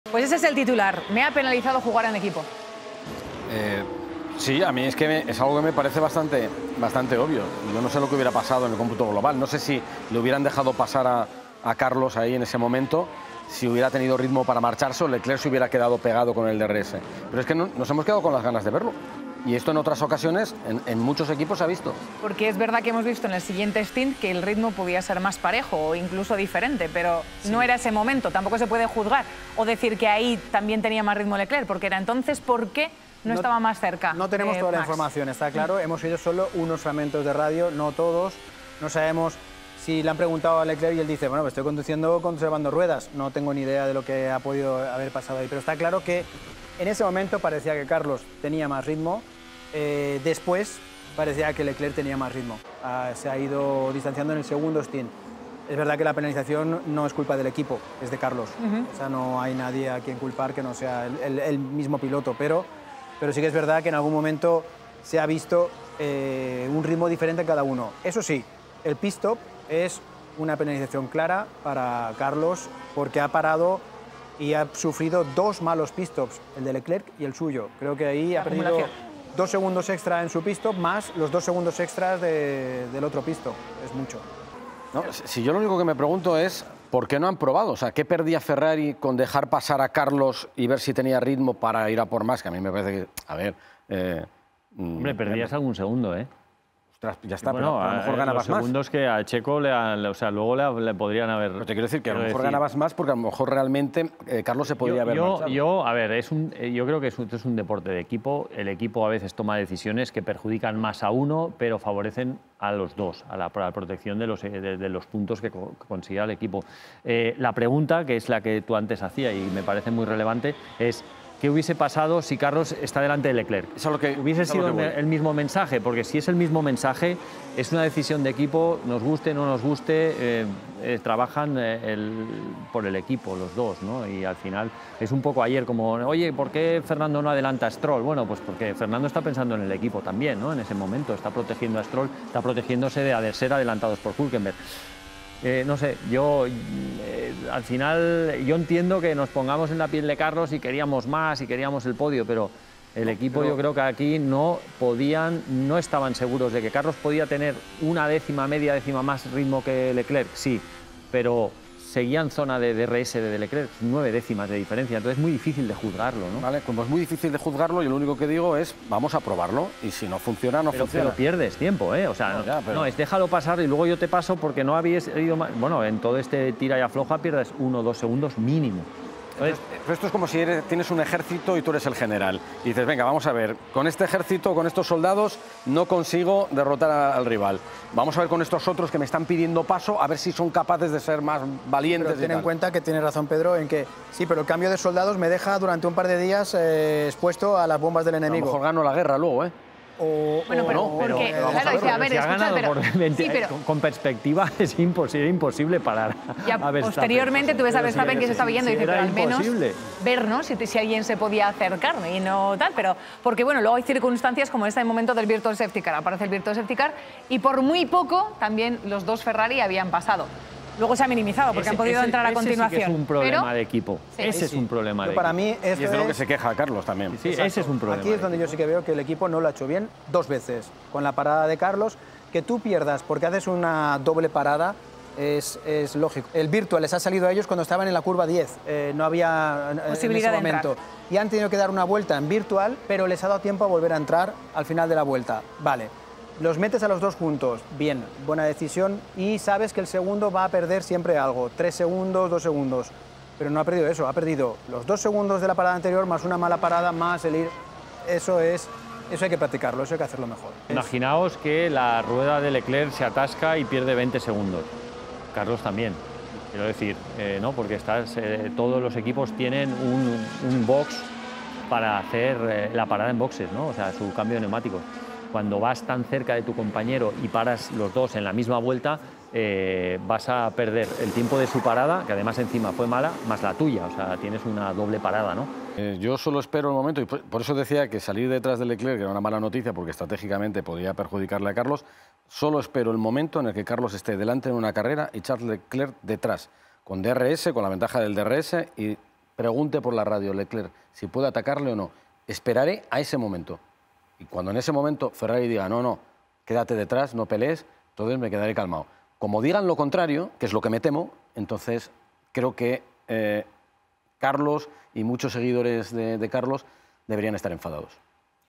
"¿Qué hubiese pasado si Sainz está delante de Leclerc?": La estrategia de Ferrari, a debate
"¿Qué hubiese pasado si Carlos Sainz está delante de Charles Leclerc en el Gran Premio de Austria de F1?". Esa pregunta lanza Pedro Martínez de la Rosa tras lo visto en el Red Bull Ring, donde el español de Ferrari pidió paso al verse con más ritmo que el monegasco. Además, él, Antonio Lobato y Toni Cuquerella analizan la estrategia de la doble parada de la Scuderia durante el Virtual Safety Car desplegado en el gp austriaco.